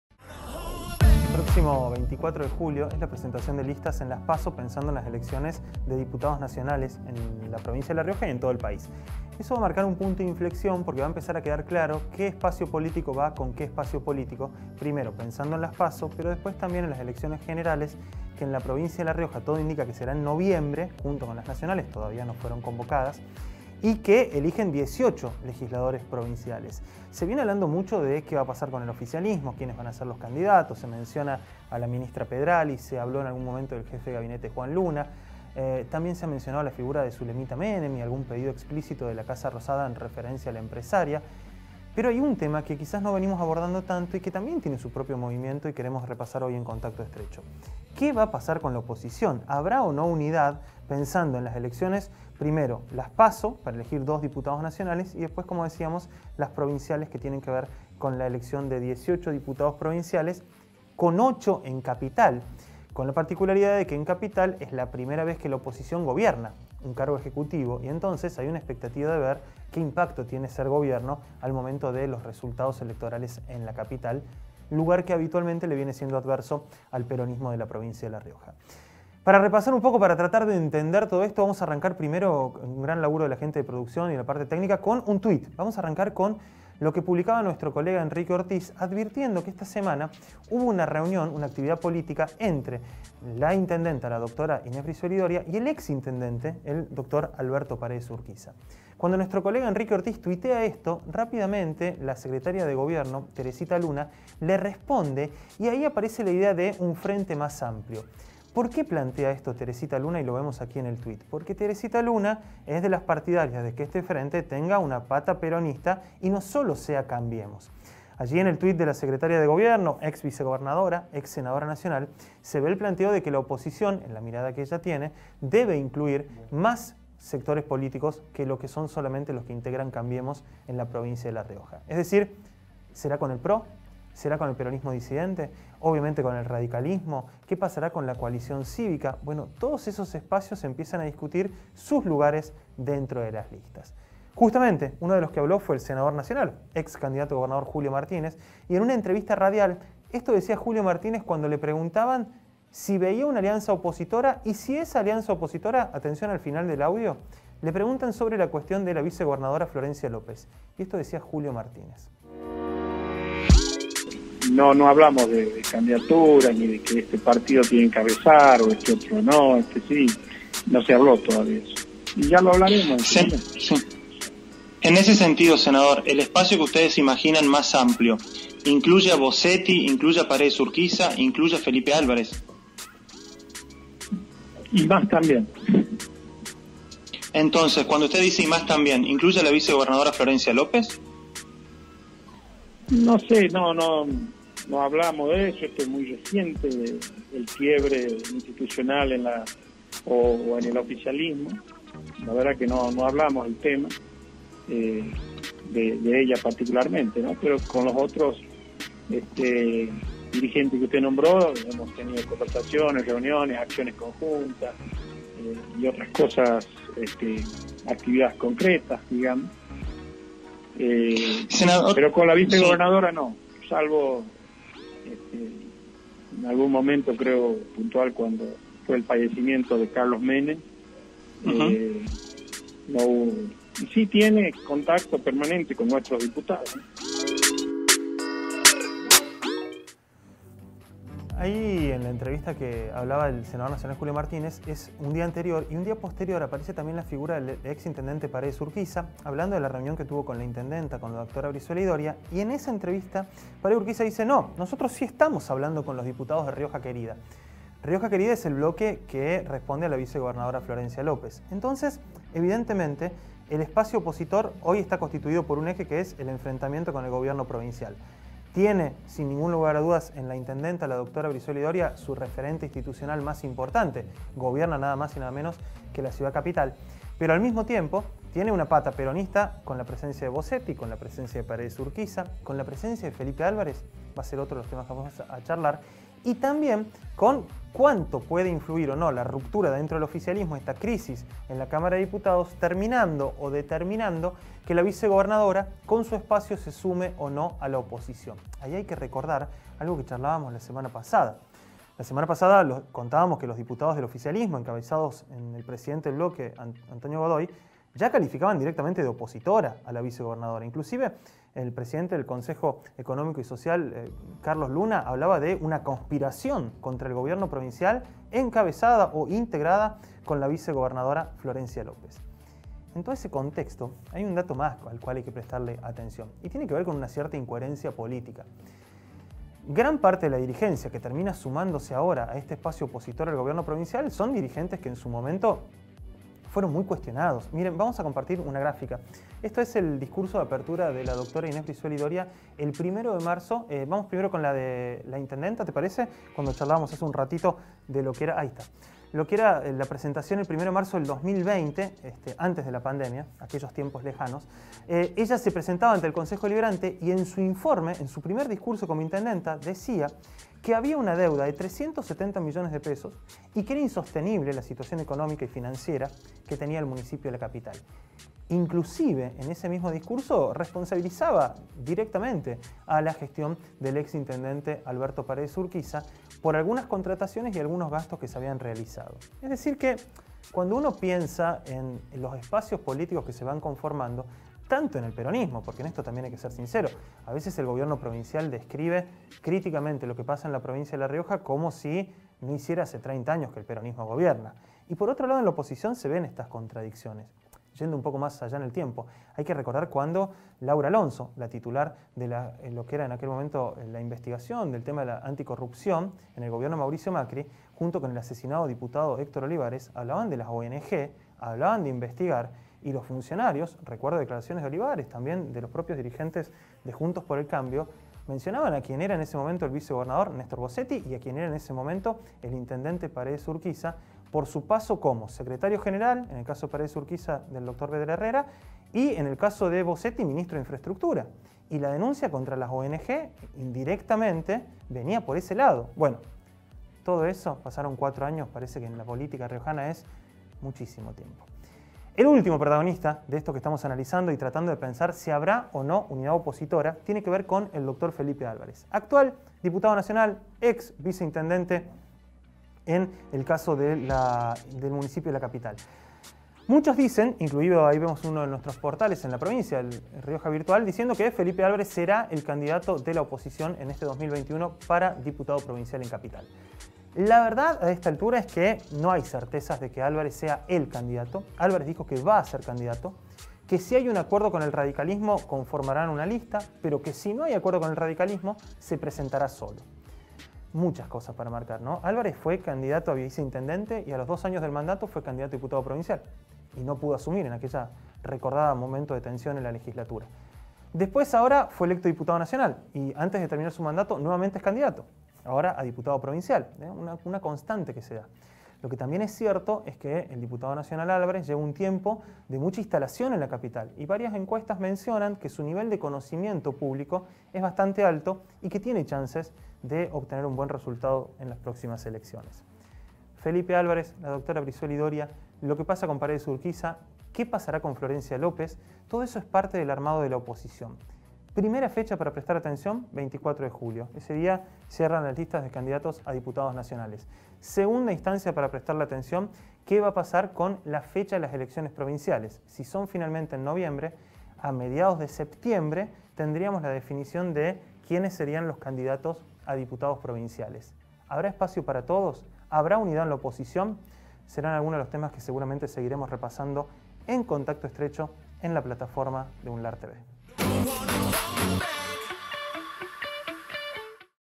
¿Habrá unidad opositora?
El periodista Mattías Meragelman analiza el escenario político riojano. La alianza que podría ser, la incoherencia política en el discurso de la oposición y el rol de Felipe Álvarez en la alianza.